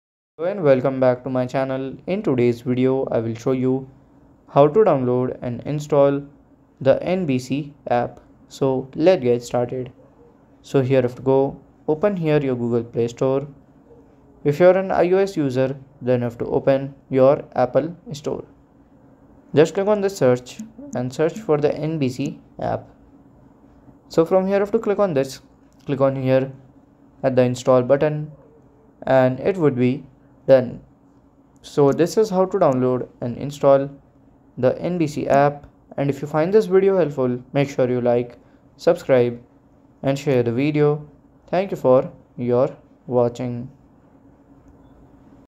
✅ How To Download and Install NBC Sports App (Full Guide)
This comprehensive guide provides step-by-step instructions for downloading and installing the app on your device. From watching live events to accessing highlights, NBC Sports offers comprehensive coverage for sports enthusiasts. Watch now and never miss a moment with the NBC Sports App!

Key Learning Points:

Step-by-step instructions for downloading and installing the NBC Sports App
Tips for navigating sports content, setting up notifications, and customizing preferences effectively
Overview of key features and sports streaming functionalities